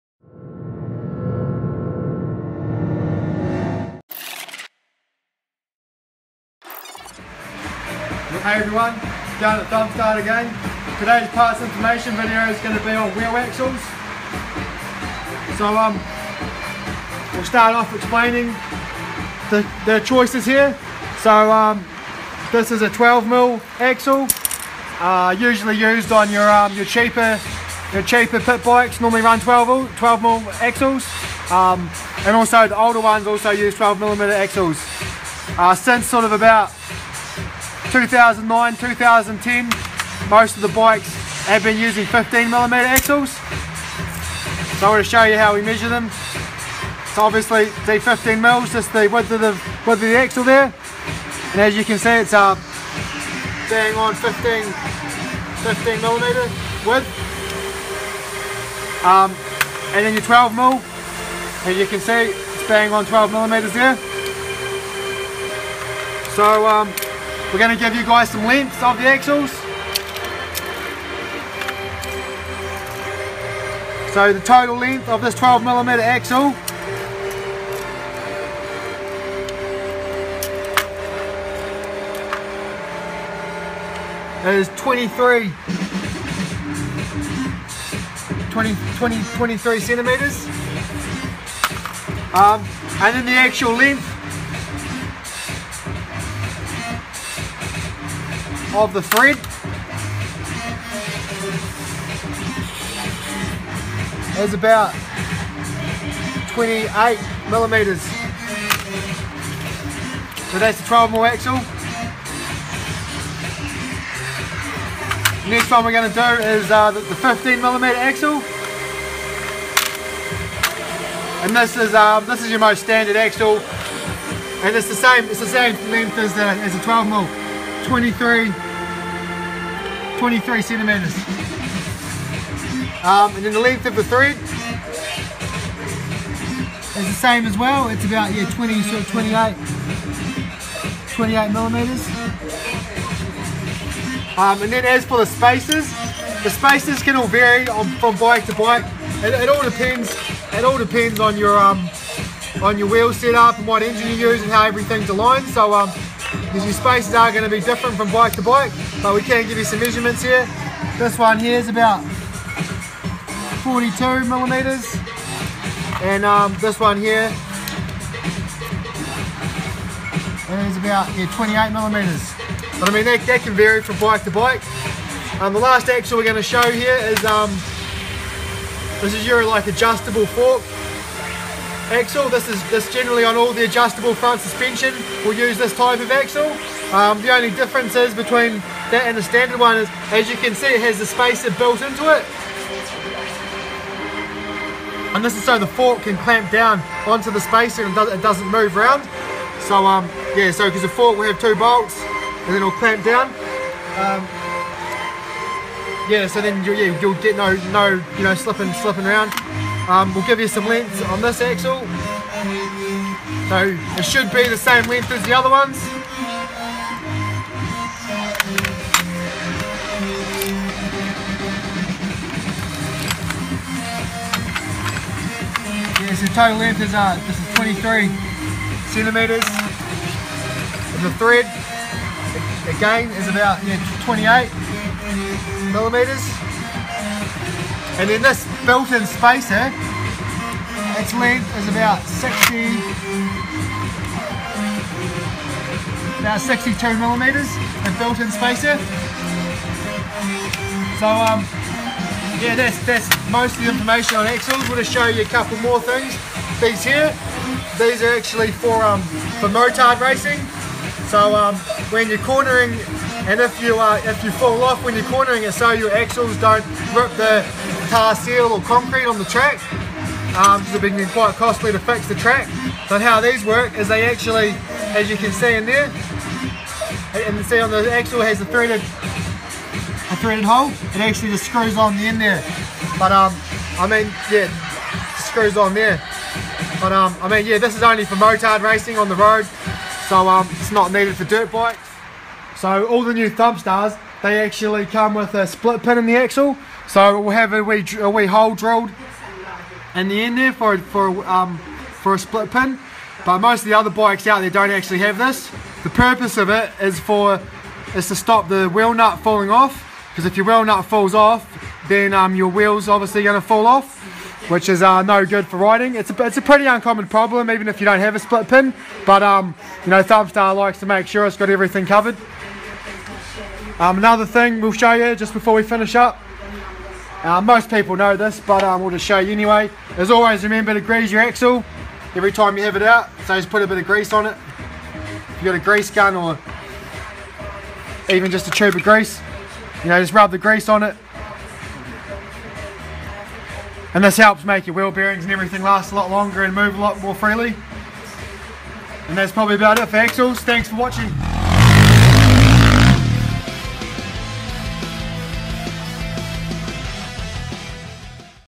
Wheel Axle Information - Thumpstar
Wheel Axle Information and Measurements.

Please note this is a information video linked to our website to help customers when purchasing parts, its not a tech video.